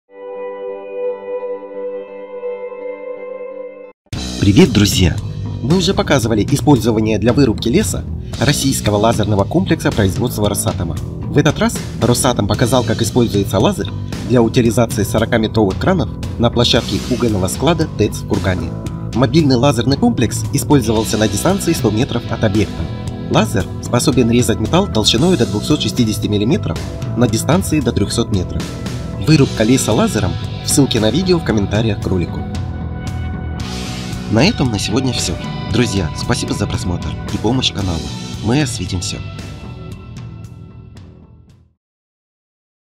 Российский лазер Росатома разрезал кран
Мы уже показывали использование для вырубки леса, российского лазерного комплекса производства «Росатома». В этот раз "Росатом" показал как используется лазер для утилизации  40-метровых кранов на площадке угольного склада ТЭЦ в Кургане. Мобильный лазерный комплекс использовался на дистанции 100 метров от объекта, лазер способен резать метал толщиной до 260 мм на дистанции до 300 метров. Вырубка леса лазером, в ссылке на видео в комментариях к ролику.